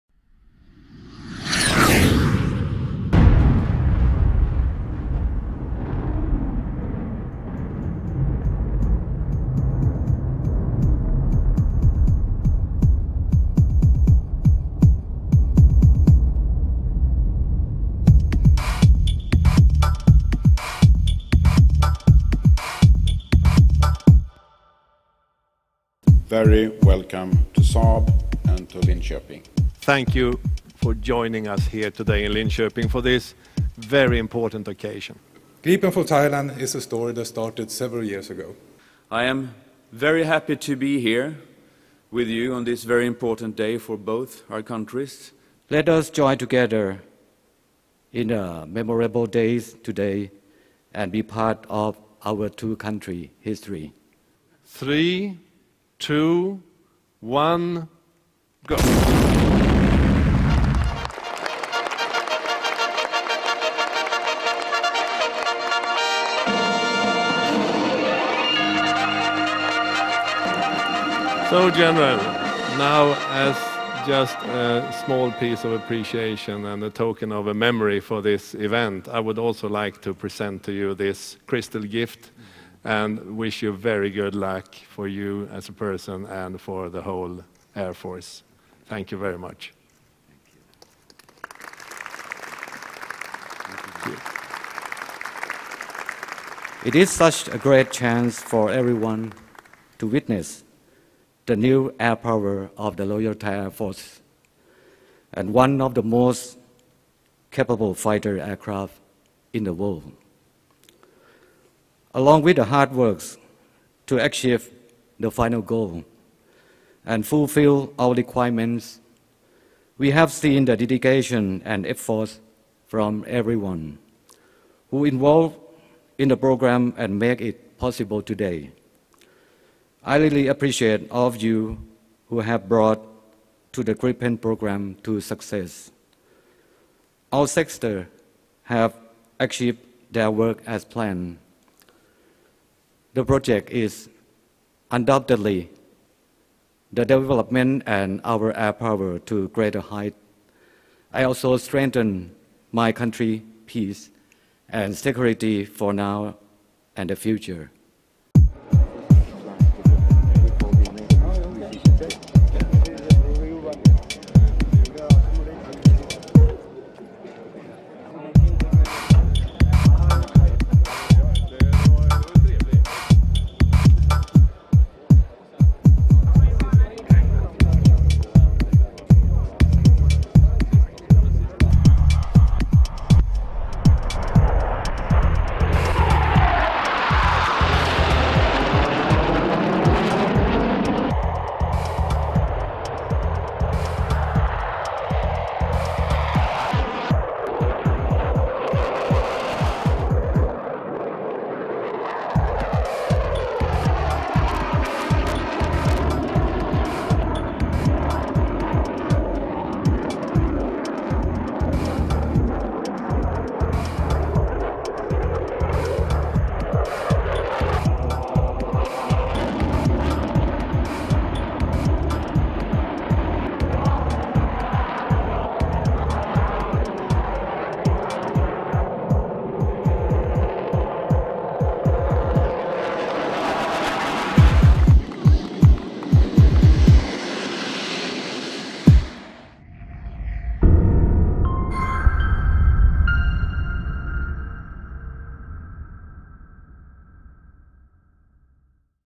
Uveiing of Thai Air Defence Solution
At a ceremony at Saab's factory in Linköping today, Saab unveiled the Thai integrated air defence system for its Thai customer. The ceremony was held to celebrate the contract between Sweden and Thailand.